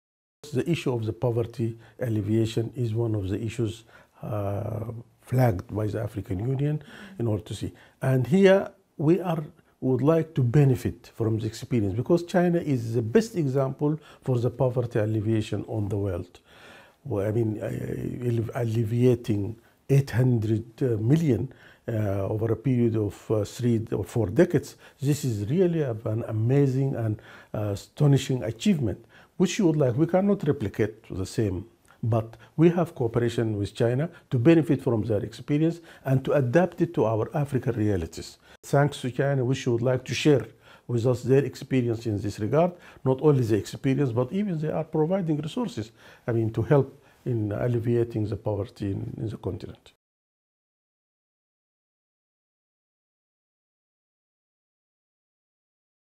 AU Ambassador: Africa can learn from China’s poverty alleviation
Global Alliance Against Hunger and Poverty expected to be launched during G20 meeting. Rahmat Allah Mohamed Osman, Permanent Representative of the African Union to China, says China's experience with poverty alleviation can be an example for Africa.